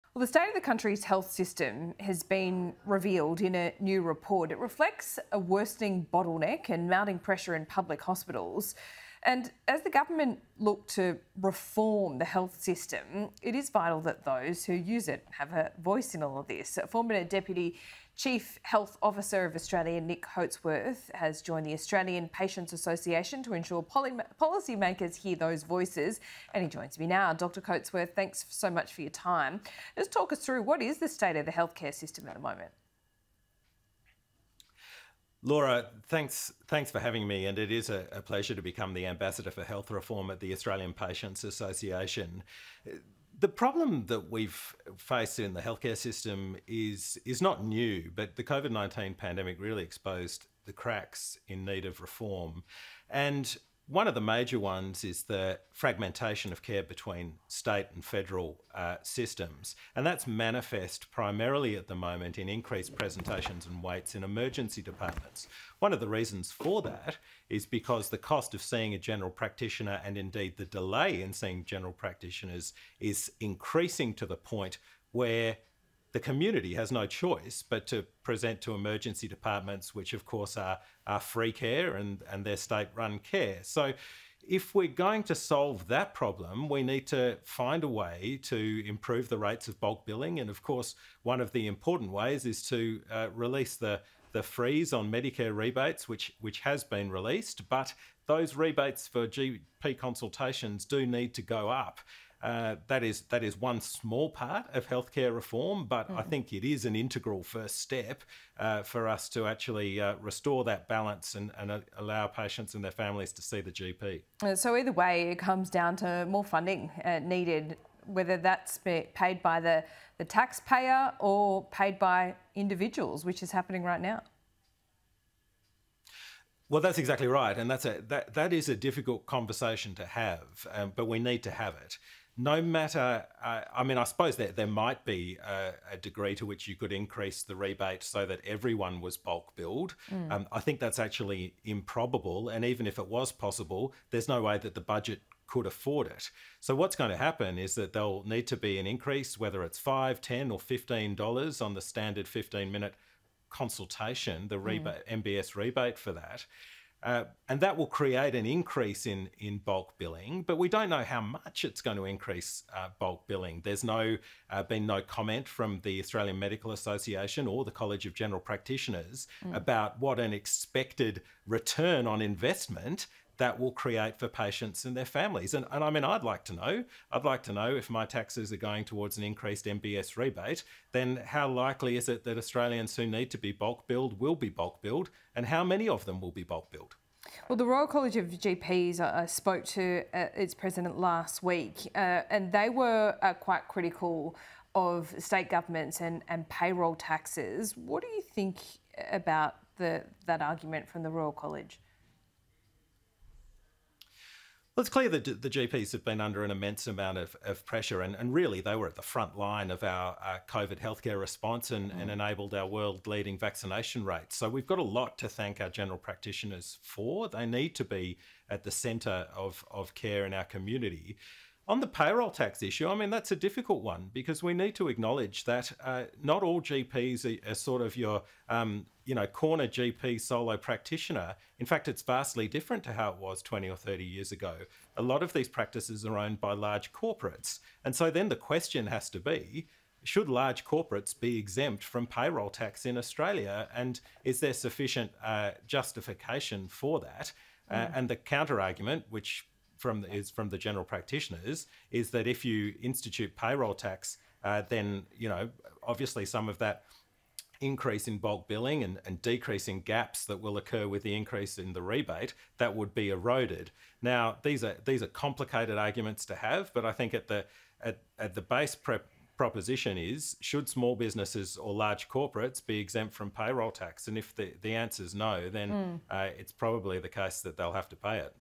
Increased GP costs mean increased admissions to emergency departments
Australian Patients Association ambassador for health reform Dr Nick Coatsworth says Medicare bulk billing rates for general practitioners must be “improved” amid an overflow of patients in emergency departments.

Dr Coatsworth said the cost and delay of seeing a GP has increased to the point where the community has no choice but to present to emergency departments.

“If we’re going to solve that problem, we need to find a way to improve the rates of bulk billing,” he told Sky News Australia.

“One of the important ways is to release the freeze on Medicare rebates which has been released but those rebates for GP consultations do need to go up.

"I think it is an integral first step for us to actually restore that balance and allow patients and their families to see the GP.”